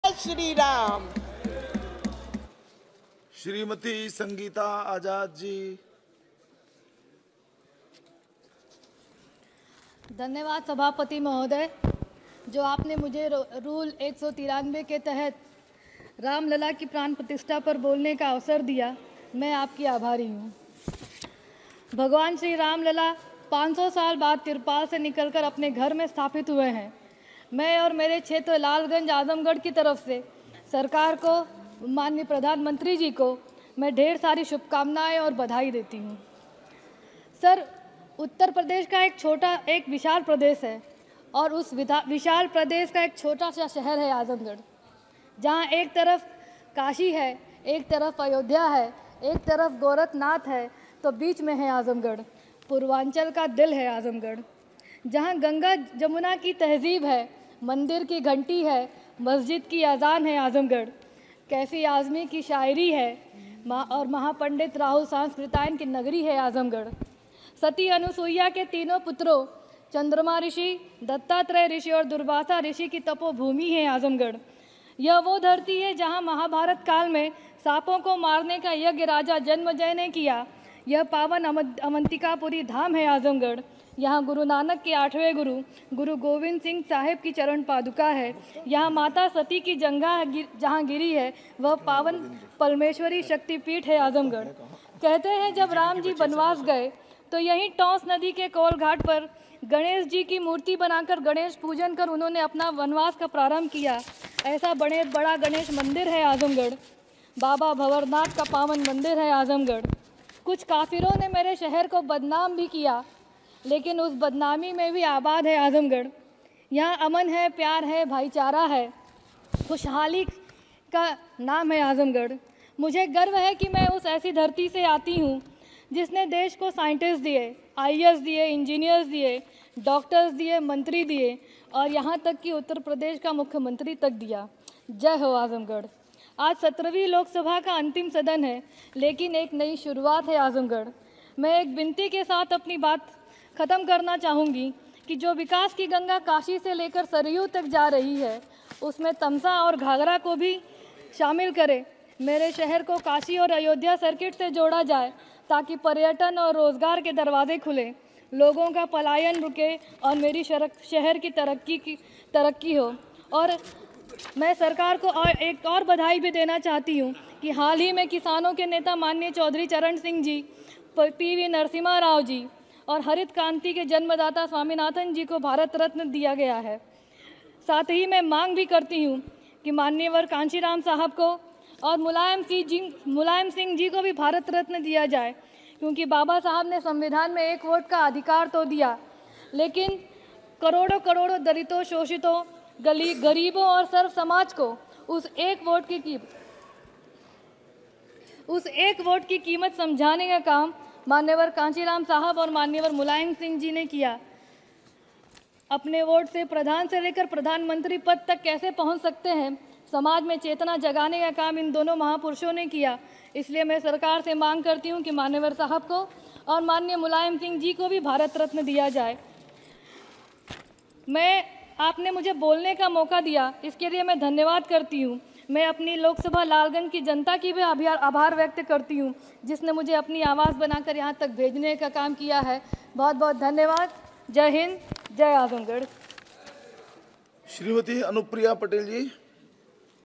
Sangeeta Azad's Remarks | Discussion on the Construction of Historic Ram Temple & Pran Partishtha
Discussion under rule 193 on the construction of the historic Shri Ram Temple and Pran Pratishtha of Shri Ramlala.